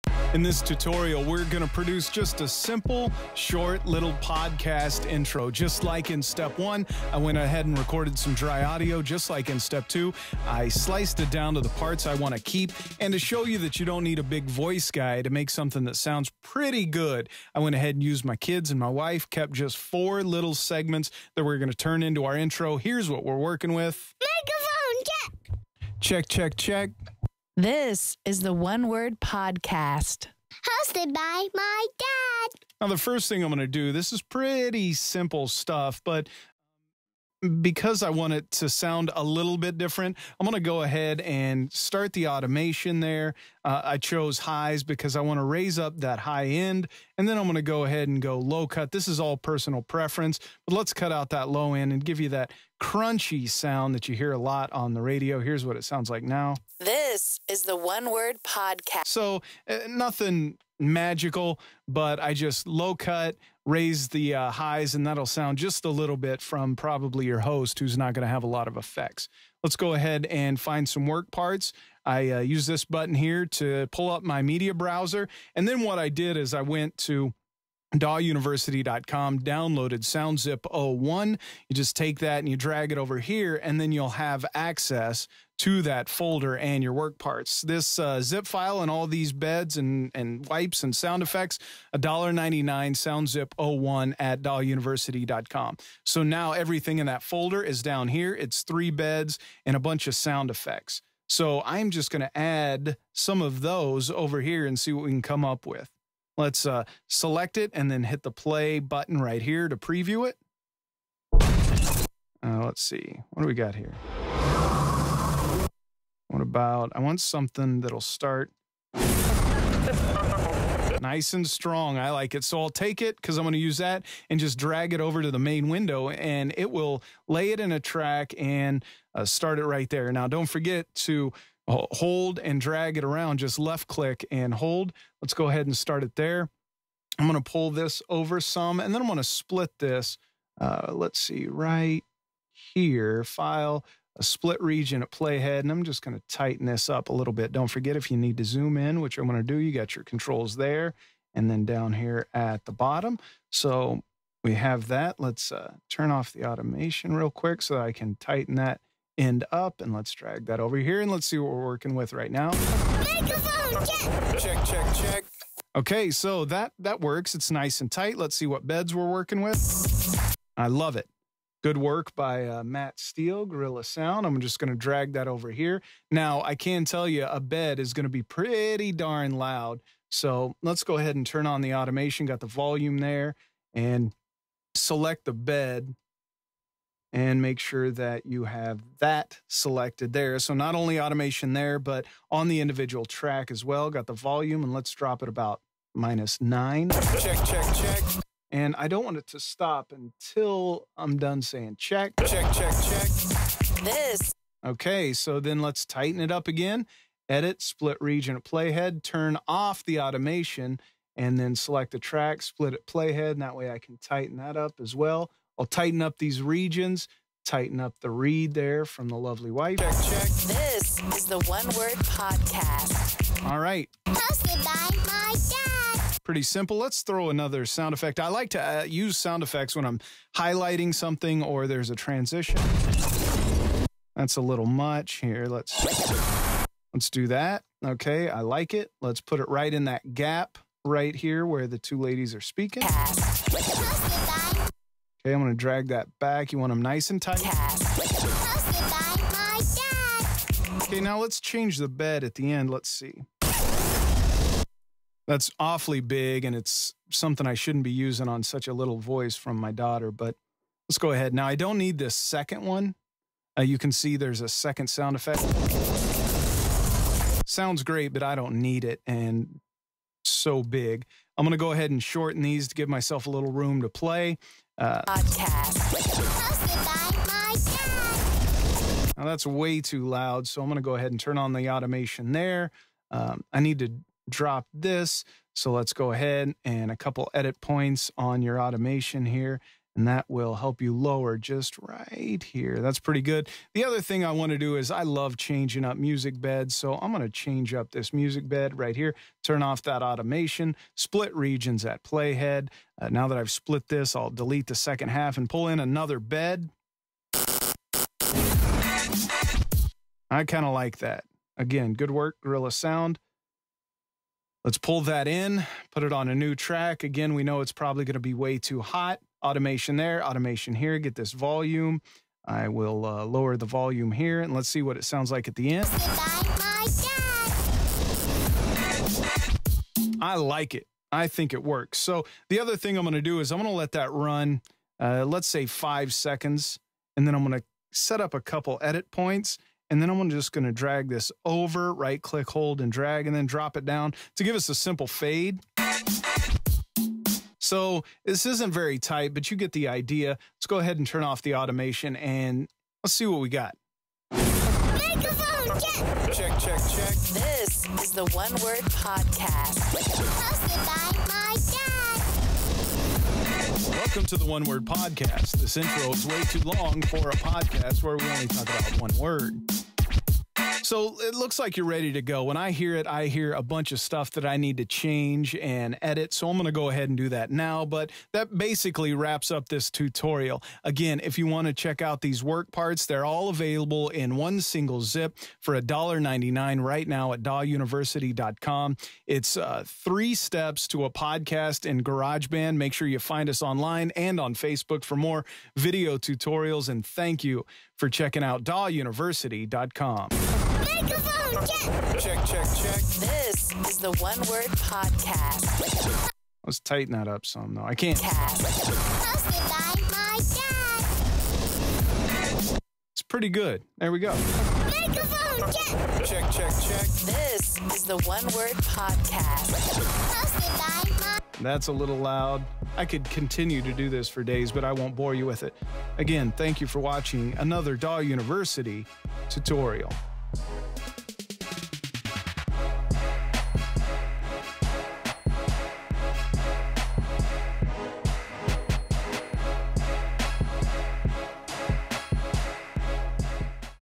Part 3 Podcasting with Garage Band, how-to produce an intro.
Part 3 of Podcasting with Garage Band in 3 steps and less than 15 minutes. In this video tutorial we will show you how to edit and produce a simple intro for your podcast with Garage Band. In Part one we recorded the audio, now we edit it and now in part three we will produce a simple intro/sweeper type thing.